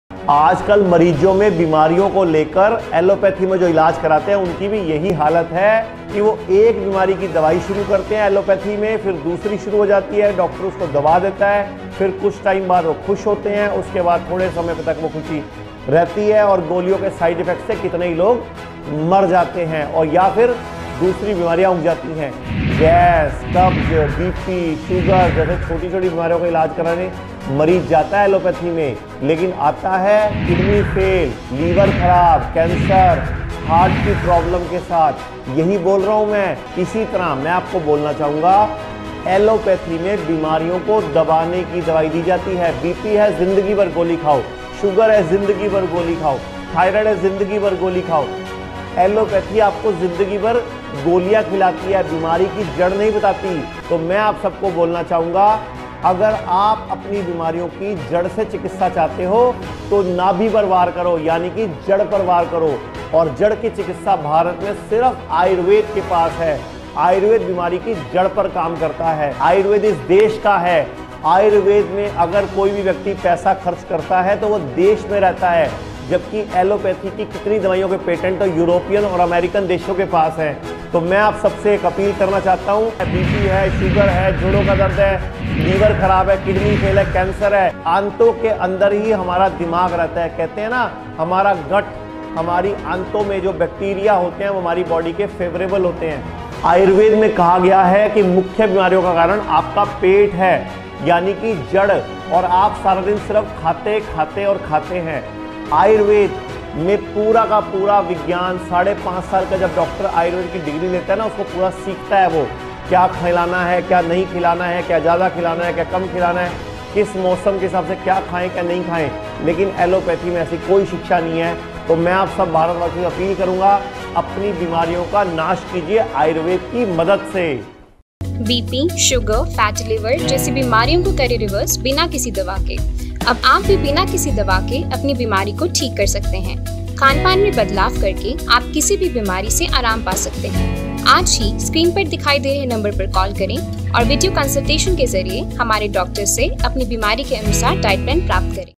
Kidney Fail | Heart Fail | liver Failure | Cancer | Dr Biswaroop Roy | BP | Hiims Chandigarh
क्या आप जानते हैं जब आप बीमार हुए थे तो एलोपैथी में इलाज के वक्त डॉक्टर ने आपको उस बीमारी के लिए गोली या दवा दी होगी। कुछ समय ठीक होने के बाद आपको दूसरी बीमारी हुई और उसके लिए आपको फिर गोलियां दी गयी। ऐसे करते-करते एक गोली से शुरू हुआ ये गोलियों का चक्रव्यूह आपको कईं ओर बीमारियों की तरफ ले जाता है।

आप जाते हो Gas, Acidity की समस्या को ठीक करने के लिए लेकिन आते हो BP, Sugar, Kidney Fail, Liver Failure Heart Fail और Cancer जैसी खतरनाक बीमारी के साथ। एलोपैथी में बीमारी को दबाने की दवाइयां दी जाती है। अगर आपको BP, Sugar या थाइरोइड की समस्या होती है तो एलोपैथी में डॉक्टर आपको ज़िन्दगी भर दवाई खाने की सलाह देता है।

यदि आपको अपनी बीमारियों को जड़ से खत्म करना है तो आपको पहले इन दवाइयों के चुंगल से निकलना पड़ेगा।  इसके लिए आपको भारत की यानी हमारी पद्दति जो आयुर्वेद है। ये बीमारी की जड़ पर जाकर काम करता है। आयुर्वेद में सही खान पान, सही दिनचर्या और जड़ी बूटियों के माध्यम से बीमारी को जड़ से खत्म किया जाता है।

यदि आप भी किसी प्रकार की बीमारी से छुटकारा पाना चाहते हैं तो आयुर्वेद को अपनाएं और इस वीडियो को इतना शेयर करें ताकि पीड़ित लोग आसानी से बीमारियों से छुटकारा पा सकें।